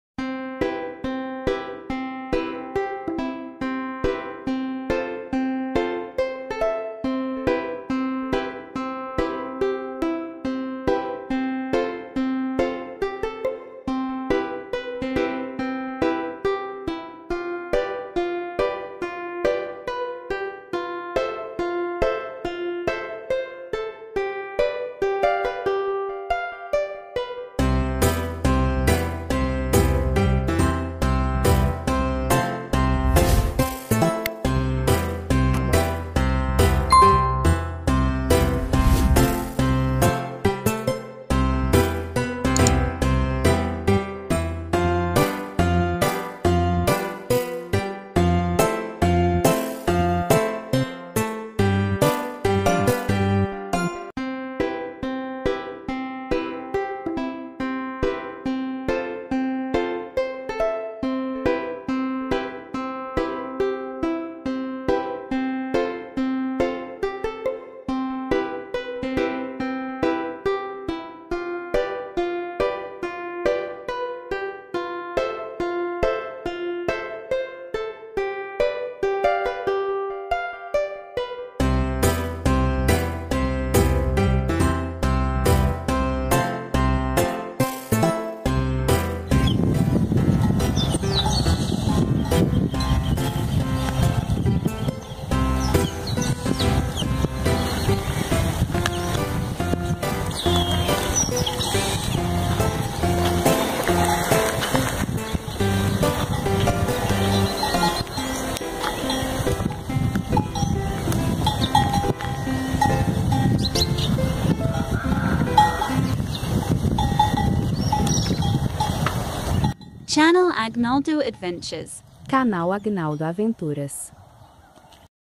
a vida dos ribeirinhos mostrando as suas hortaliças 🌱🥬 e as criações 🐓🐤🐄em Ibotirama Bahia 🇧🇷
mostrando as hortaliças e as criações 🐓🐤🐄 em Ibotirama Bahia